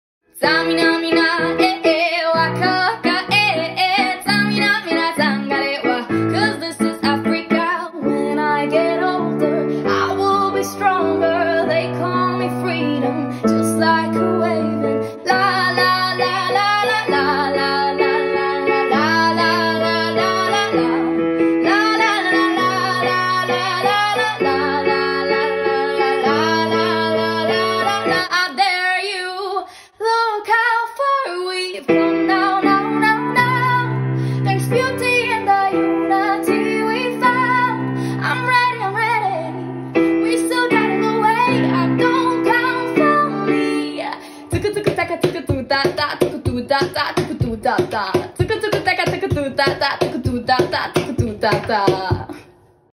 World Cup Mashup 🌎⚽️
Here's a quick mashup of World Cup Songs! Hope you like it :)